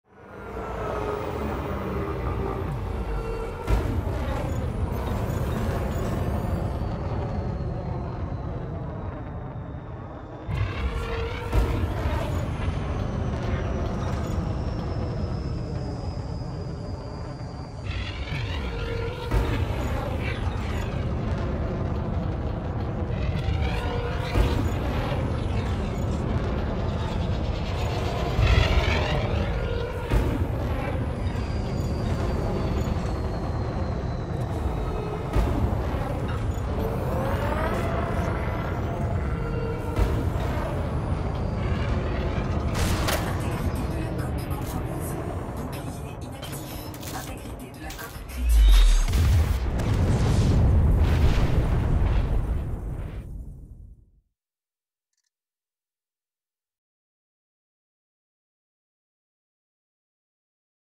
End of night Madness with Cmdr Beerz
End of night madness... Chasing down Cmdr Beerz (Quad Elite) who's determined to kill me without even deploying any hardpoints... damn son!